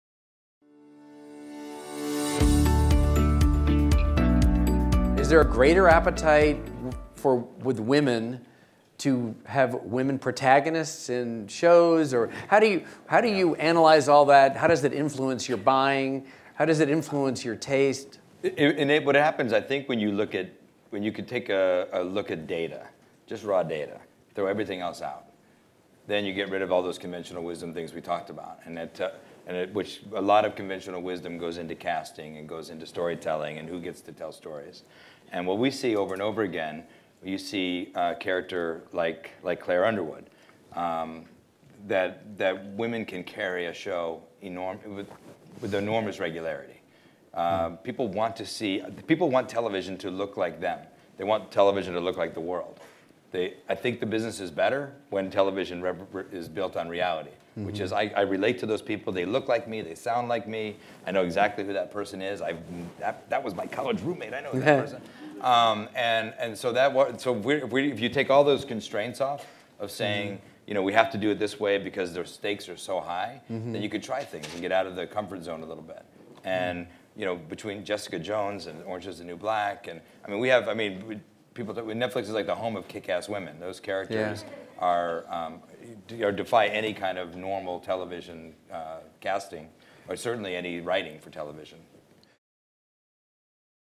Brian Grazer and Ted Sarandos On The Power Of "Kick-Ass Women"
Brian Grazer and Ted Sarandos discuss why it makes good business sense to produce content starring characters who are strong, "kick ass women".